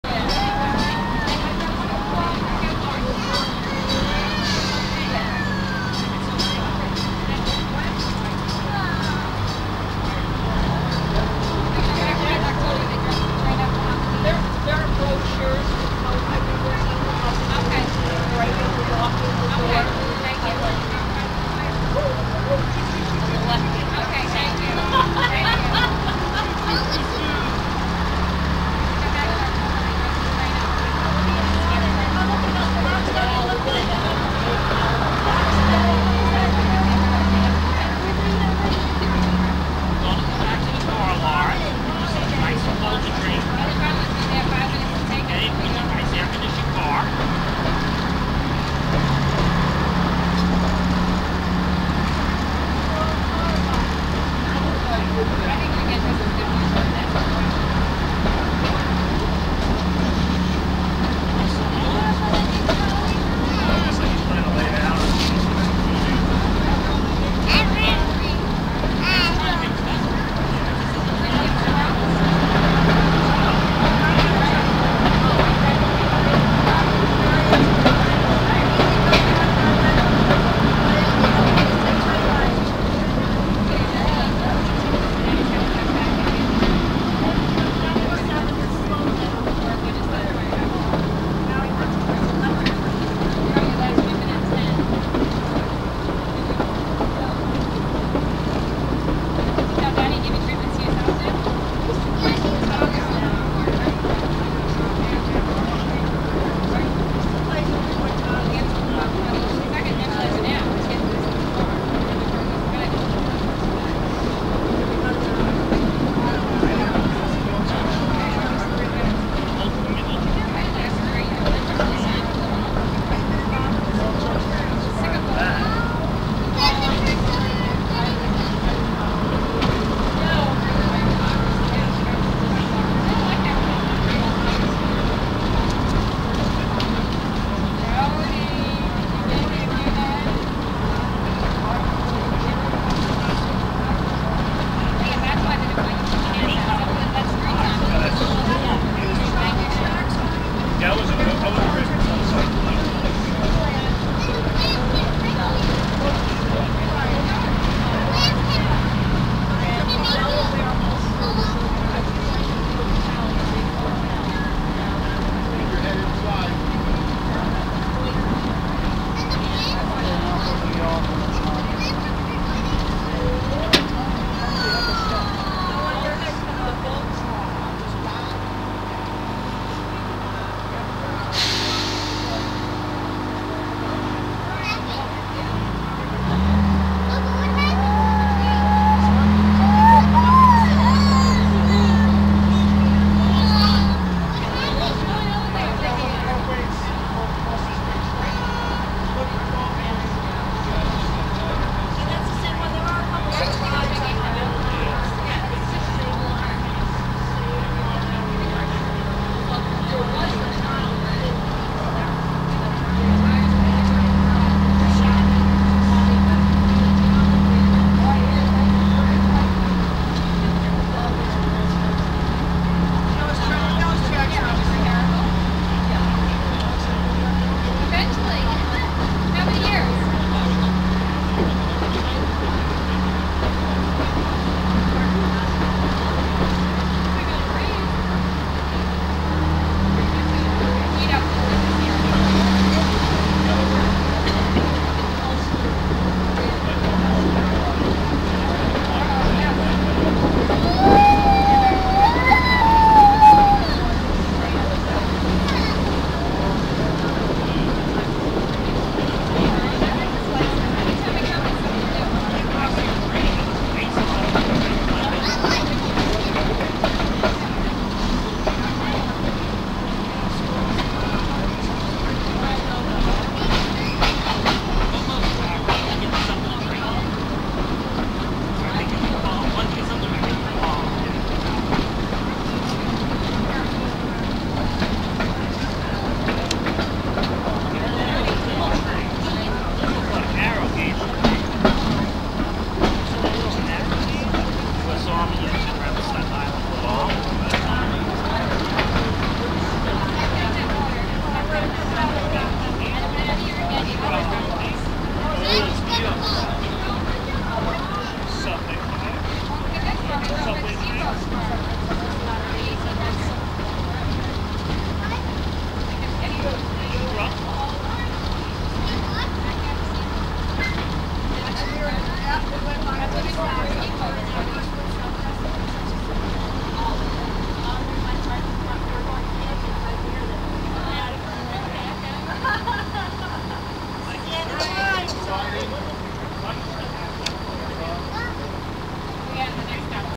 Ride on Johnstown Trolley car 358 from North St to TMNY
Ride on Johnstown Trolley car past some a junkyard along E Strand Rd and into the TMNY grounds.